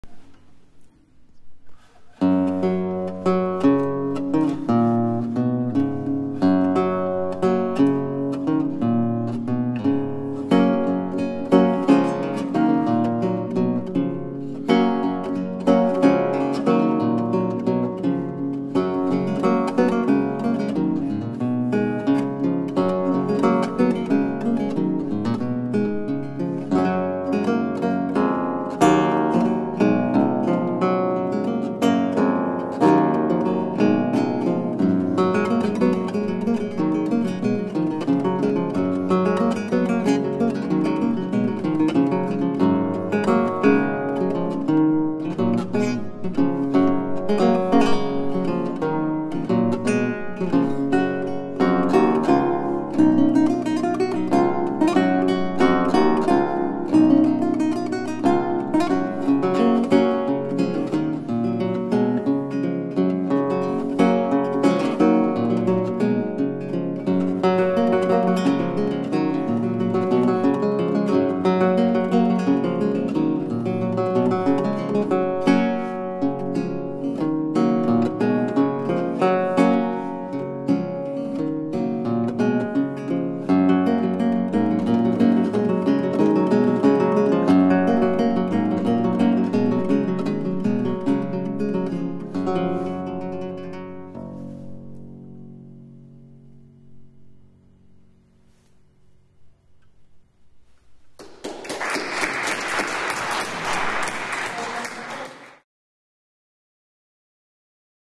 Matt Nisbet - Chaconne
Matt Nisbet performing a Chaconne in All Saints Church, Weston-super-Mare

One of 3 pieces that he performed on the theorbo in February 2013